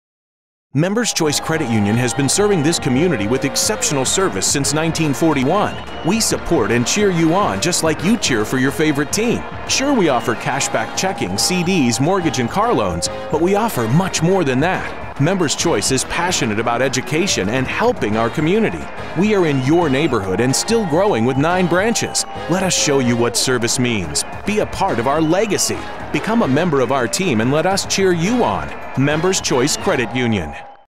Members Choice Credit Union :30 Video Spot - 2017
Members Choice Credit Union's 30 second video spot that is shown at the Katy ISD Football Stadiums - 2017